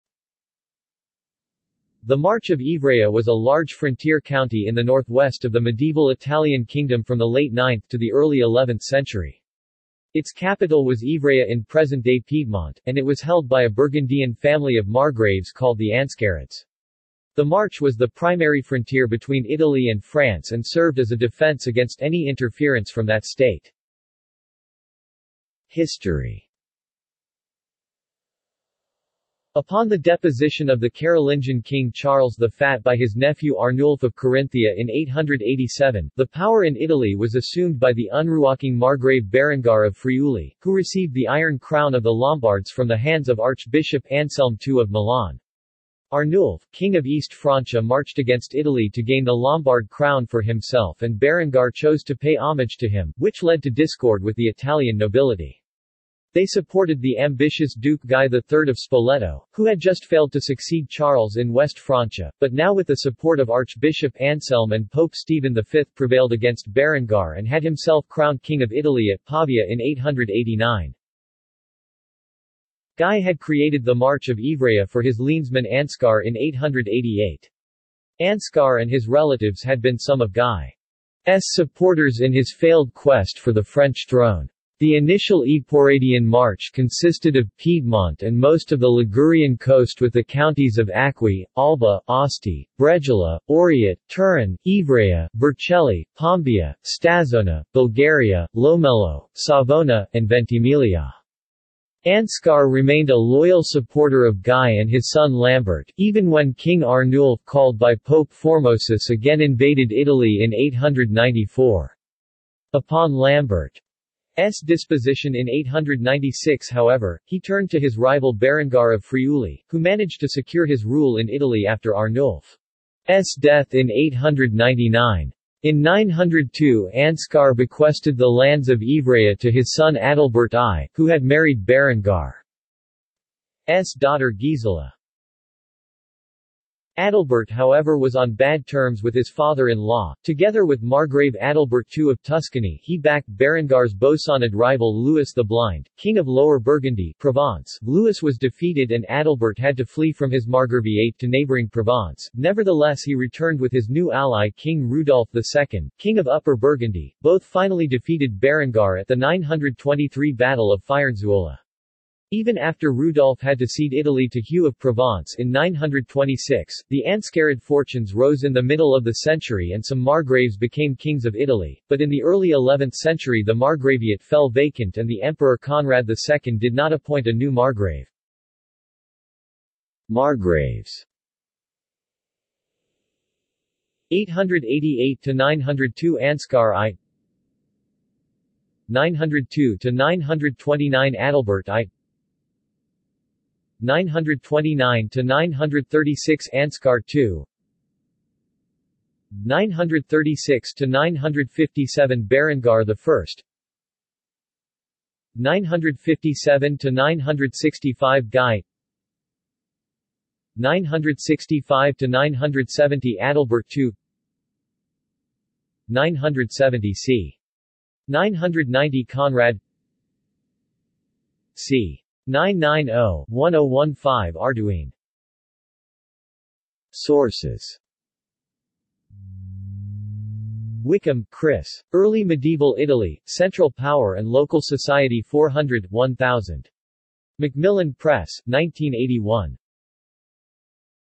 March of Ivrea | Wikipedia audio article
This is an audio version of the Wikipedia Article:
March of Ivrea

SUMMARY
The March of Ivrea was a large frontier county in the northwest of the medieval Italian kingdom from the late 9th to the early 11th century.  Its capital was Ivrea in present-day Piedmont, and it was held by a Burgundian family of margraves called the Anscarids. The march was the primary frontier between Italy and France and served as a defense against any interference from that state.